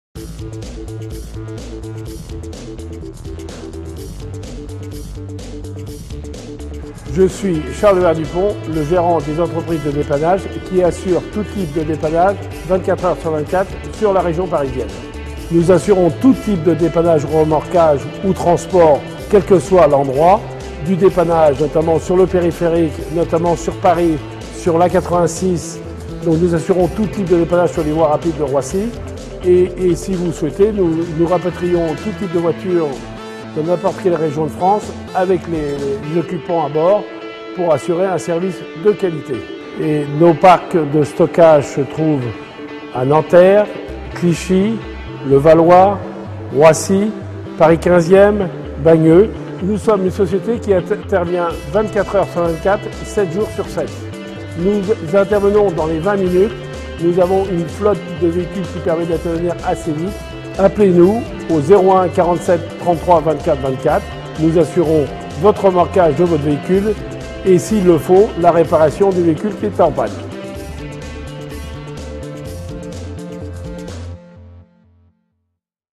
Société M Assistance est spécialisée dans le dépannage à Paris 15e
Située à Paris dans le 15ème arrondissement, la société M. Assistance est spécialisée dans le dépannage et le transport de véhicules et de motos.